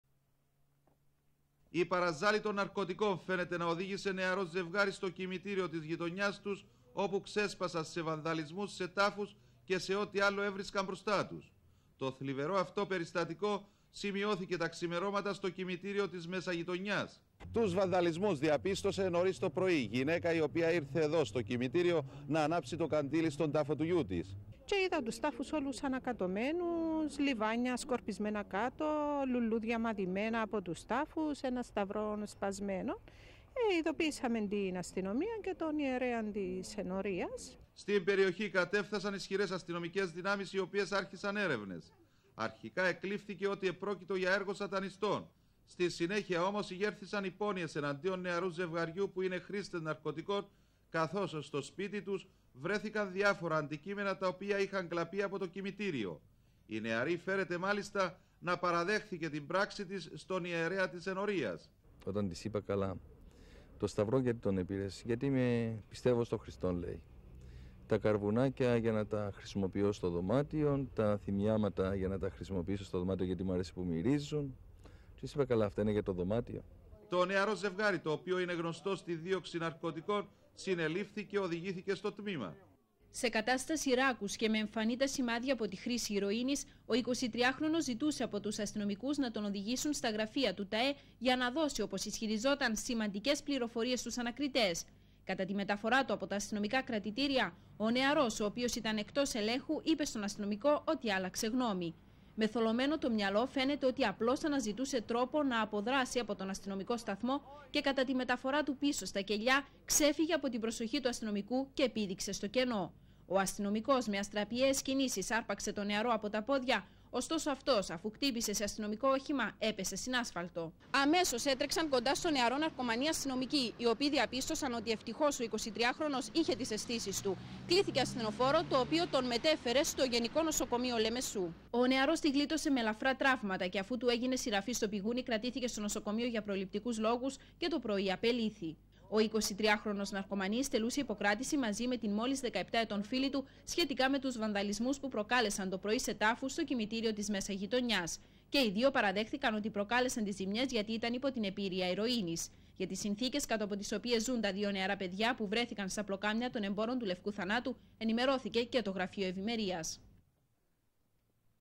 Τον Ιανουάριο 2002, νεαρό ζευγάρι ναρκομανών προκάλεσε βανδαλισμούς στο κοιμητήριο Μέσα Γειτονιάς
Ρεπορτάζ: Πάμπος Μιτίδης και Κατερίνα Χαραλάμπους
Κάμερα: Κλείτος Ερωτοκρίτου
Μοντάζ: Ανδρέας Μιχαηλίδης
Παραγωγή: Videoscope studio